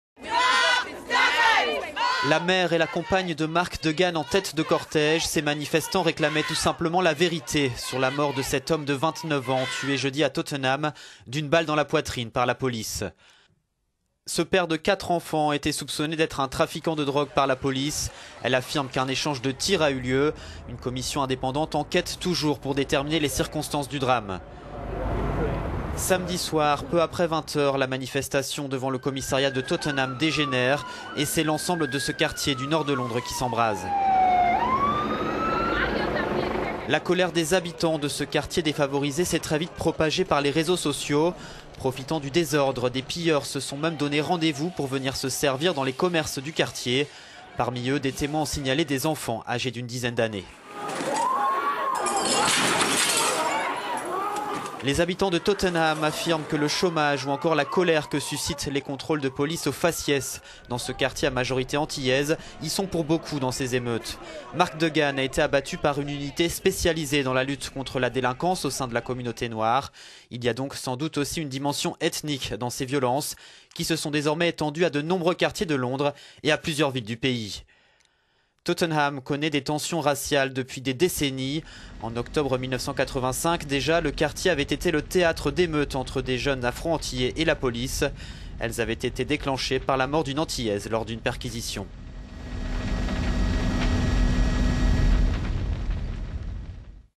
Tottenham: les raisons de la colère
La mère et la compagne de Mark Duggan en tête de cortège, ces manifestants réclamaient tout simplement la vérité sur la mort de Mark Duggan, un homme de 29 ans tué jeudi à Tottenham d'une balle dans la poitrine par la police.

Ce père de quatre enfants était soupçonné d'être un trafiquant de drogues par la police. Elle affirme qu'un échange de tir a eu lieu. Une commission indépendante enquête toujours pour déterminer les circonstances du drame.